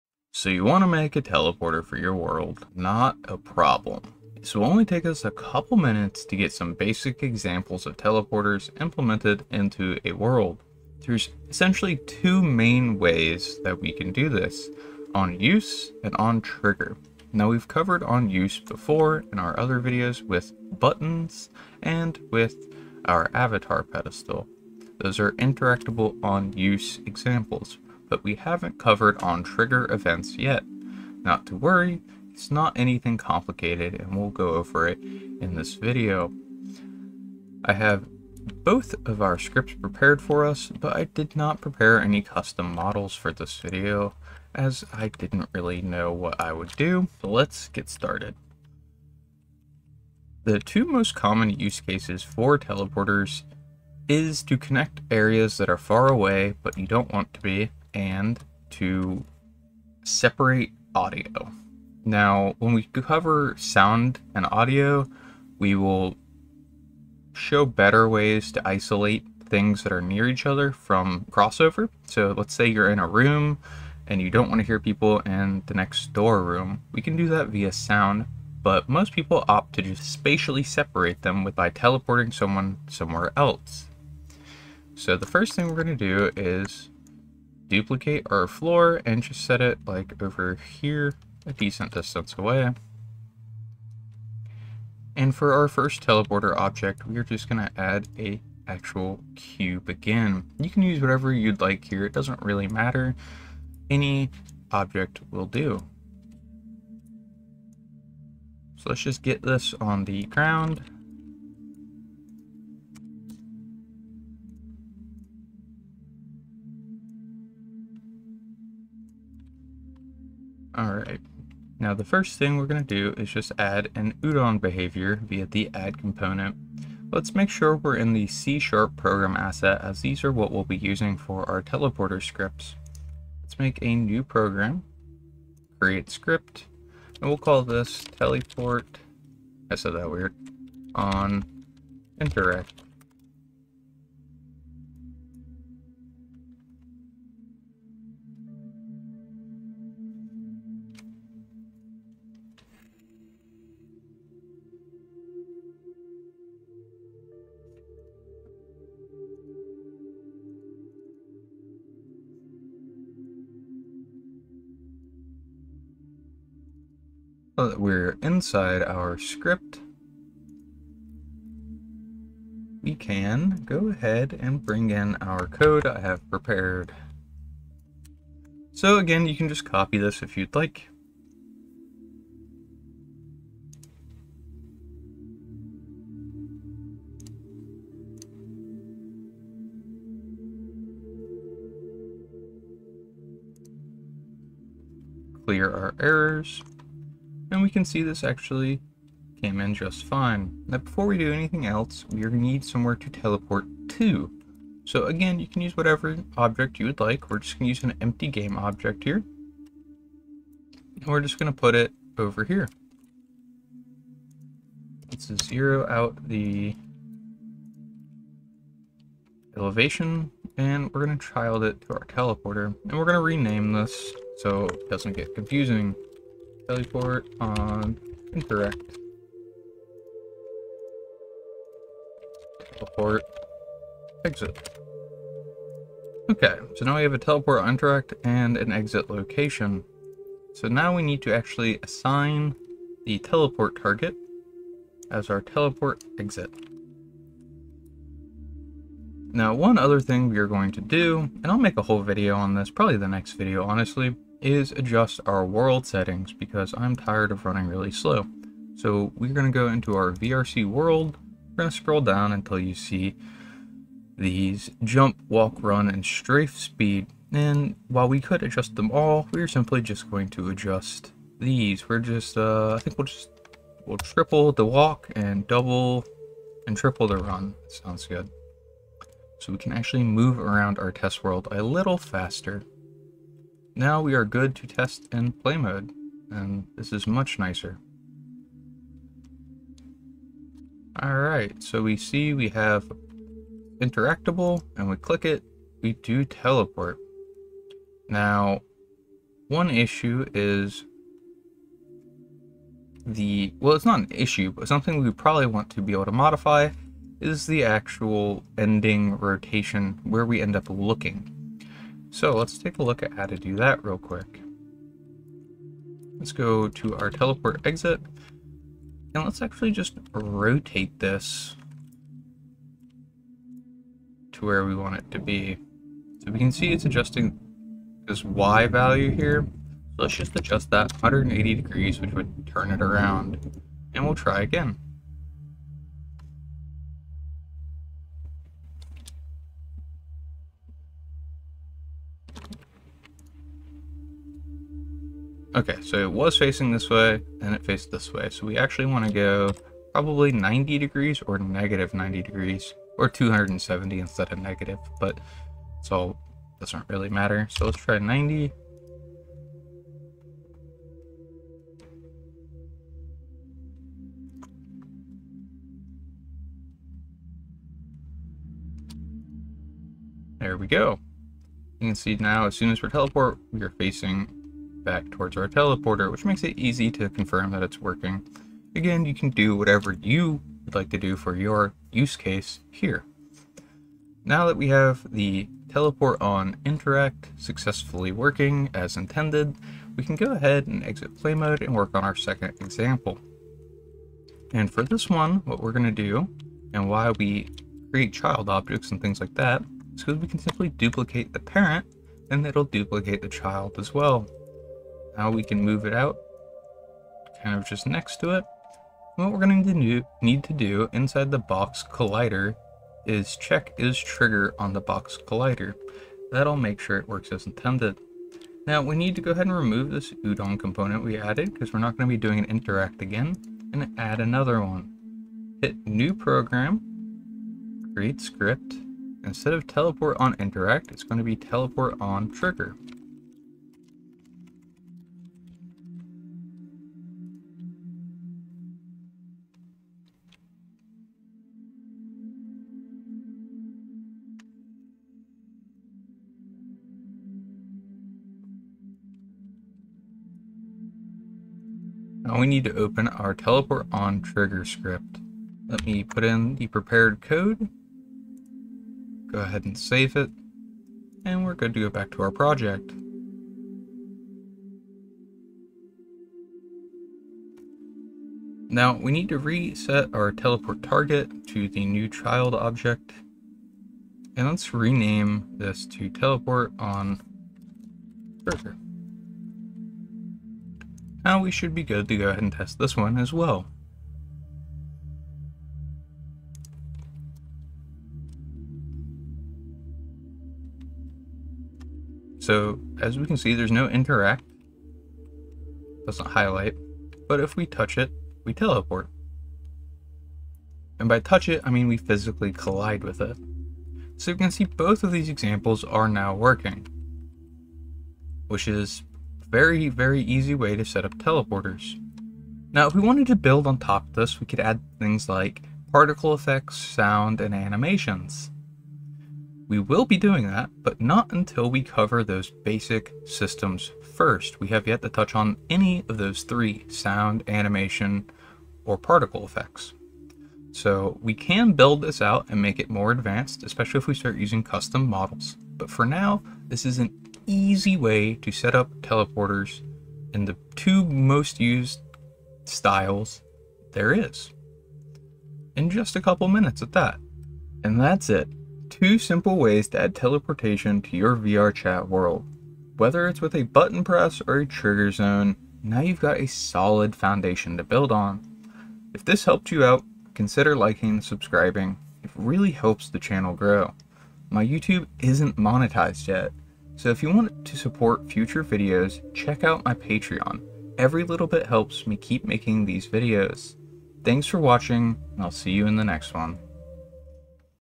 VRChat World Tutorial: How to Make Teleporters
In this VRChat world tutorial, I’ll show you how to make two types of teleporters: one activated by a button, and one triggered when a player walks into a zone. This is a beginner-friendly guide using UdonSharp and the VRChat SDK, perfect for creators just getting started with world building.

No complex systems, no extra tools — just clear, straightforward setup and scripting.